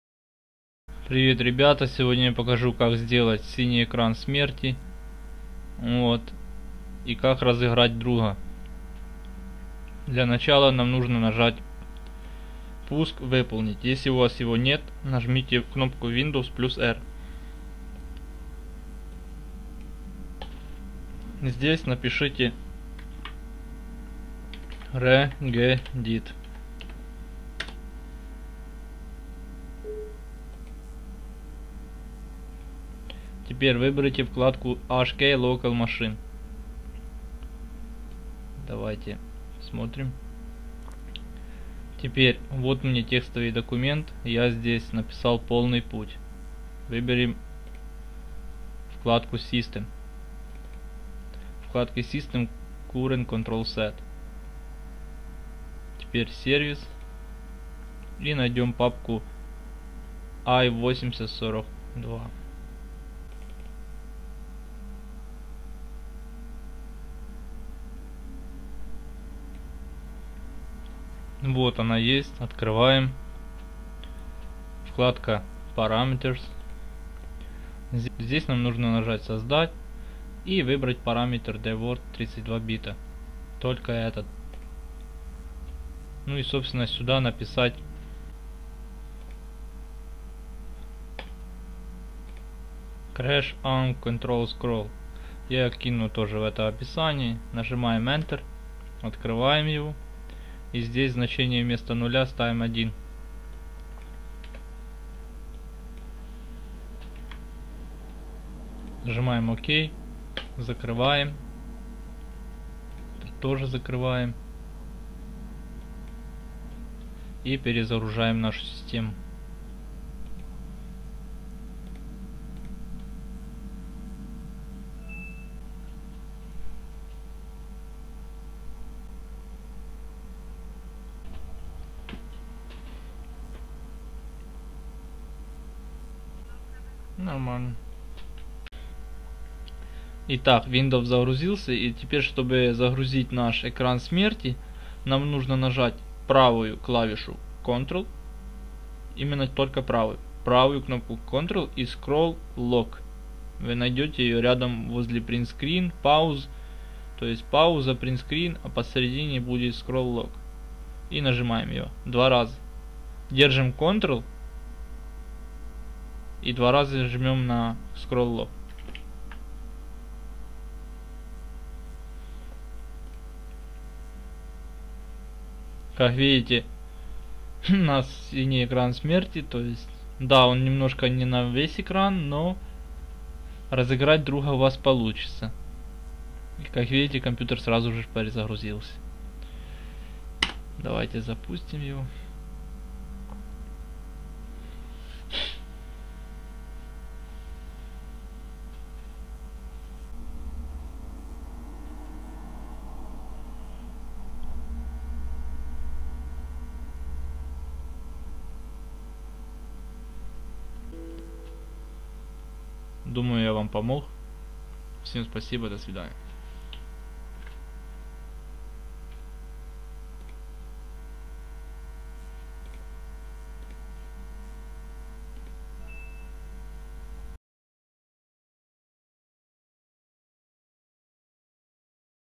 Как запустить Синий Экран смерти вручную, розыгрыш друзей
Telegram канал Компьтерная Помощь Вызываем синий экран смерти Windows
В выполнить вписываем команду regedit
HKEY_LOCAL_MACHINE\SYSTEM\CurrentControlSet\services\i8042prt\Parameters
В реестре пишем CrashOnCtrlScroll

Иногда в практических целях необходимо сгенерировать критическую ошибку BSOD (Blue Screen Of Death)
Для выяснения возможных потерь данных, действий программ автоматического оповещения сисадмина, поведения различных периферийных устройств бывает необходимо вызвать критическую ошибку BSOD (Blue Screen Of Death - синий экран смерти  ▬▬▬▬▬▬▬▬▬▬▬▬▬▬▬▬▬▬▬▬